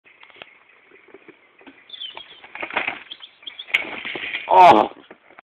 Faceplant bmx
my freind getting proper owned